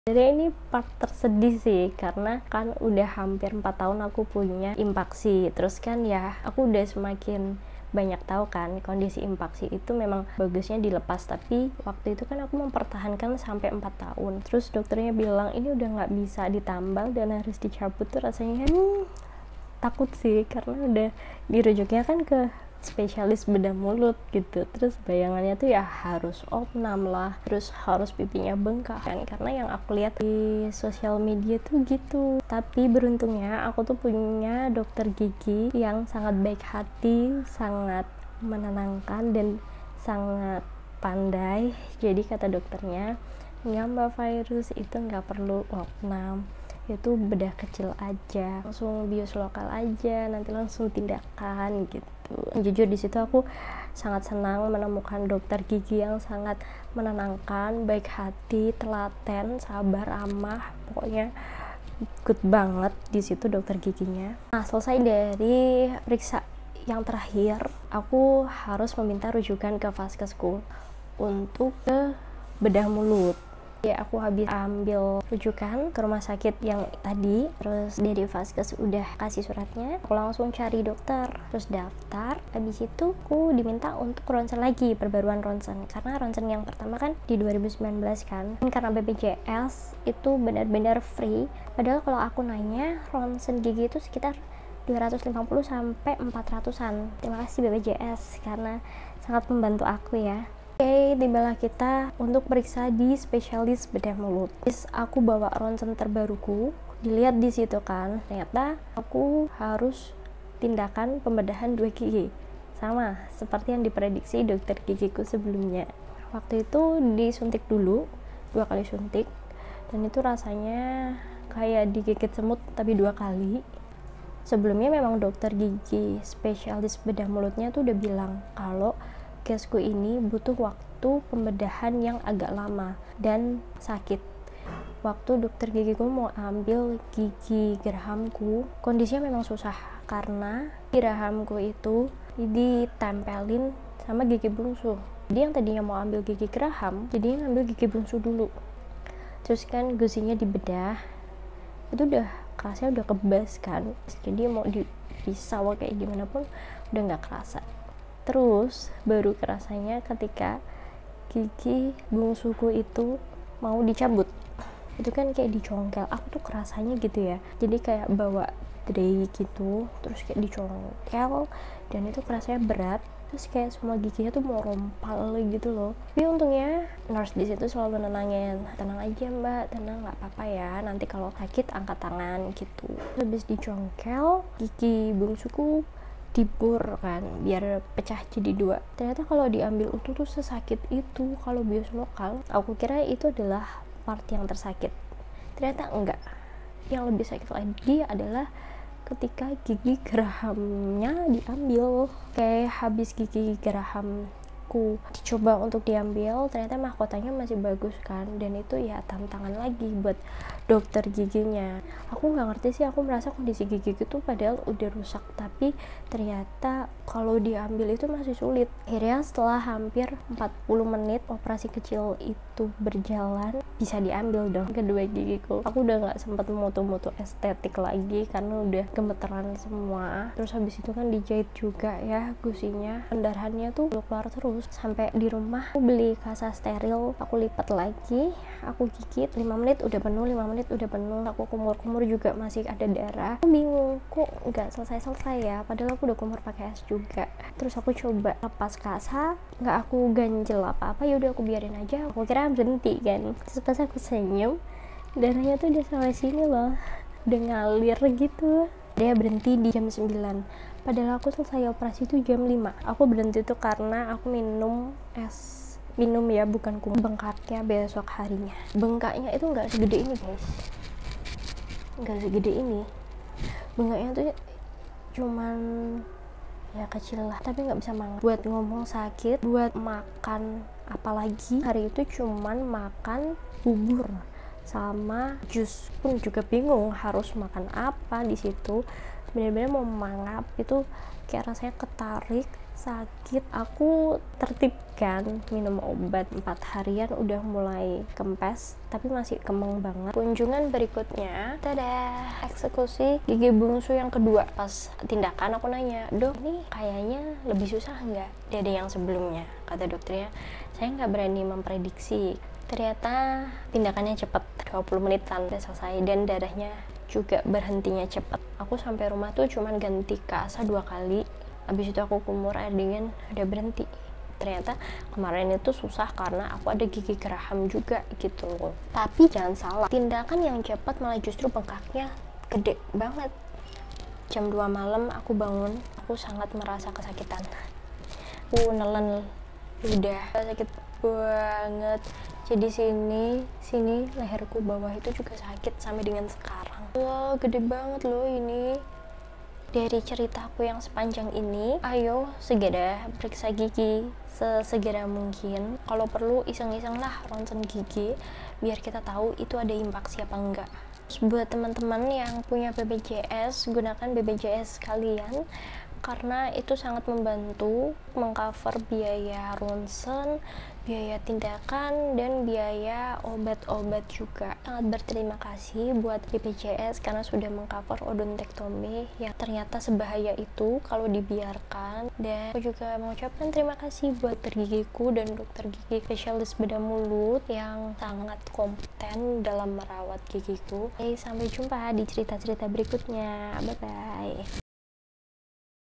PART 2 CABUT GIGI BUNGSU (ODONTEKTOMI) : RASANYA DIBEDAH...
Halo, odontektomi memang tidak pernah diharapkan kehadirannya. Tapi, jika kamu mengalami, yuk segera ambil tindakan bedah. Memang syakit, tapi setelah itu lega~~